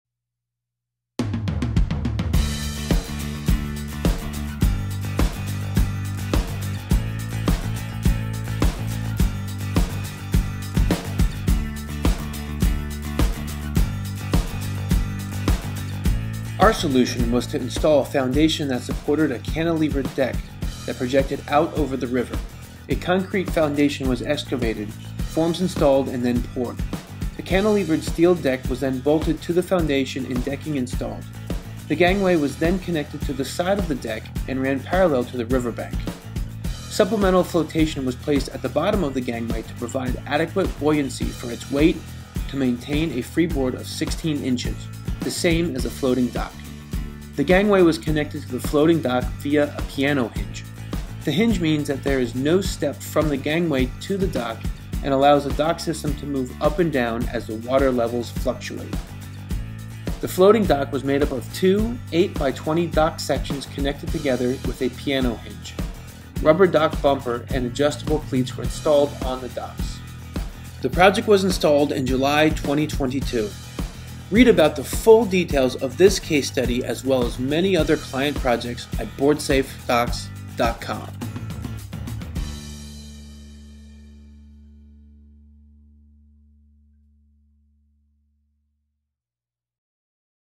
Aluminum Floating Docks and Gangway at Riverside Dock
BoardSafe Docks was contracted to come up with a design for a new floating dock system on the Schuylkill River across from Pottstown, PA, in North Coventry Township. The site included an existing boat ramp and signs of a previous dock system that was washed away in a flood years ago. The customer asked if we could design a new dock system that would hold up during flooding conditions.  It had to be ADA compliant and not project too far into the river.

The BoardSafe solution was to install a foundation that supported a cantilevered deck that projected out over the river. A concrete foundation was excavated, forms installed and then poured. The cantilevered steel deck was then bolted to the foundation and decking installed. The gangway was then connected to the side of the deck and ran parallel to the river bank. Supplemental flotation was placed at the bottom of the gangway to provide adequate buoyancy for its weight and to maintain a freeboard of 16-inches, the same as the floating dock. The gangway was connected to the floating dock via a piano hinge. The hinge means there is no step from the gangway to the dock and allows the dock system to move up and down as the water levels fluctuate. The floating dock was made up of two 8’ x 20’ dock sections connected together with a piano hinge. Rubber dock bumper and adjustable cleats were installed on the docks.

The components of the dock system included:

Foundation- 8’ x 10’ x 4’ with rebar cage and drive steel pilings
Cantilevered deck- 8’ x 20’, Steel construction, steel railing and composite decking
Gangway- 5’ x 54’ with expanded section at top
Floating Dock- 8’ x 40’, manufactured in two sections

For more information about this project and others visit BoardSafeDocks.com